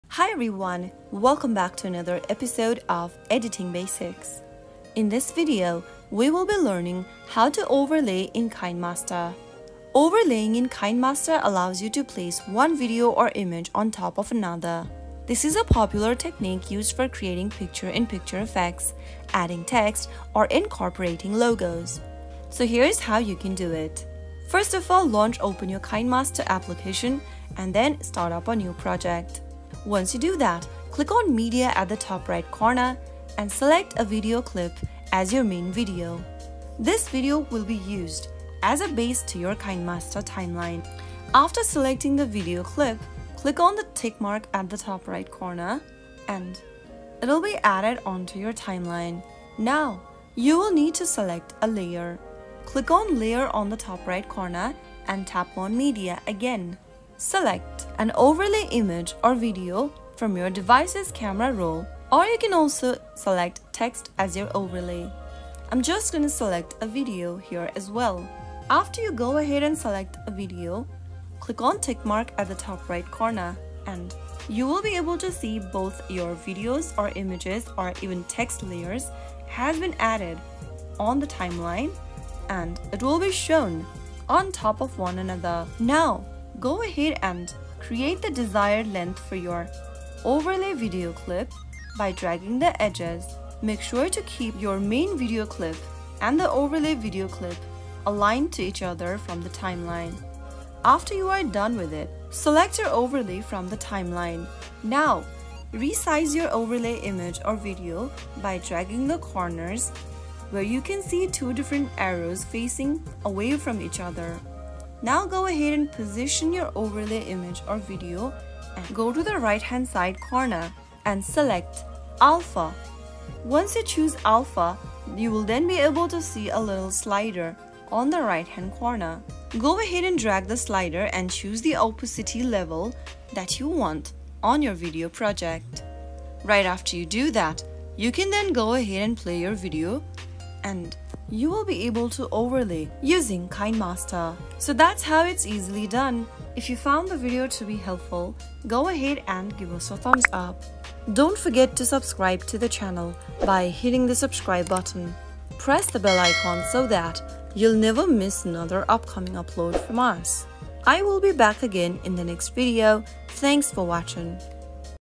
How to Add Overlay in Kinemaster 2024?
In this video, I'll guide you through the steps to add an overlay in Kinemaster. Overlays are a powerful tool to enhance your videos, allowing you to add additional graphics, images, or text layers on top of your main video.

By the end of this video, you'll know how to effectively add and customize overlays in Kinemaster to make your videos more engaging and visually appealing.